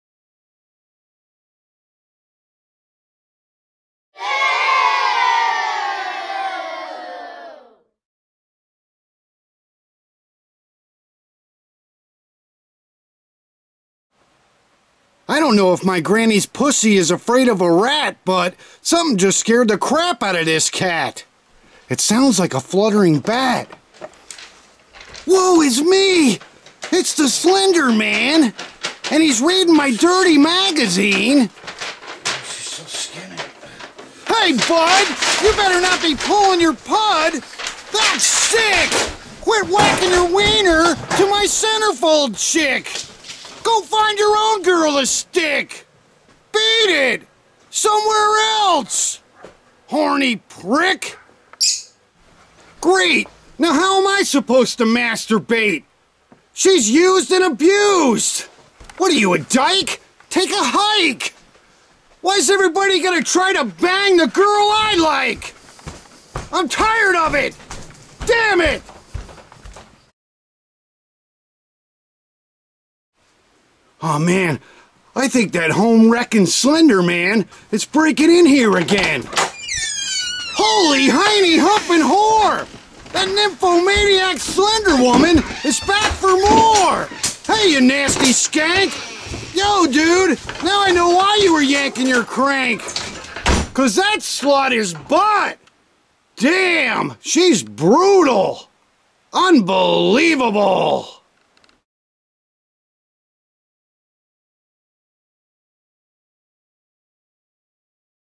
HORRIFYING SLENDER MAN SIGHTING
Shocking encounters with the Slender man captured on video .